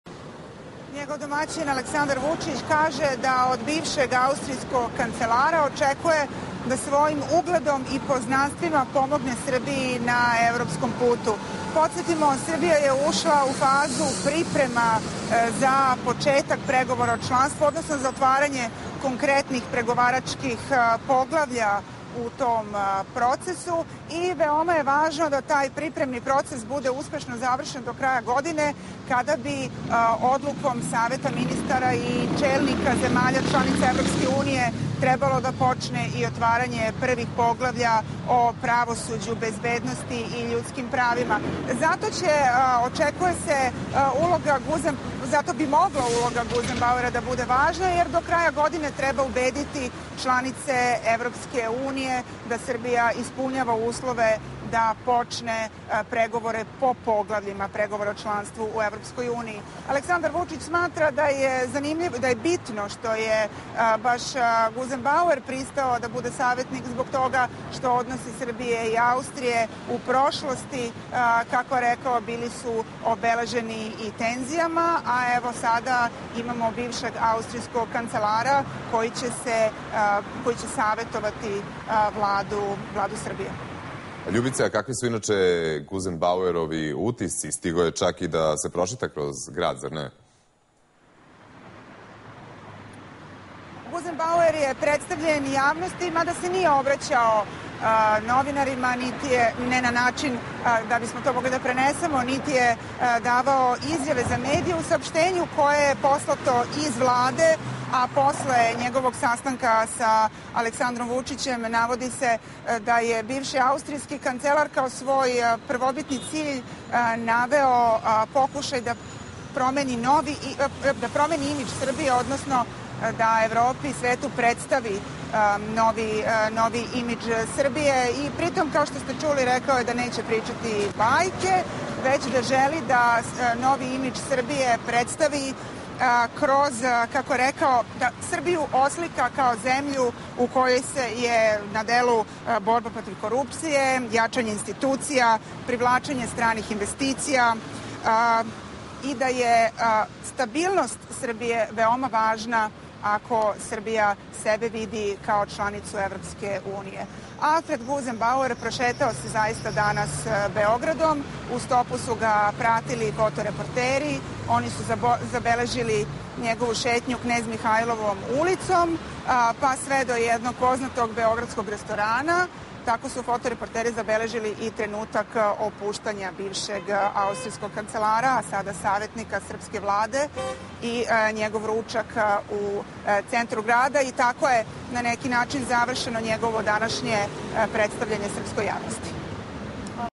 Guzenbauer: Stvaraćemo novu sliku
Bivši austrijski kancelar i savetnik nekoliko svetskih kompanija Alfred
Guzenbauer stigao je u Vladu i sastao se sa njenim prvim potpredsednikom
Aleksandrom Vučićem. On će ubuduće savetovati Vladu, naročito na polju
evrointegracija.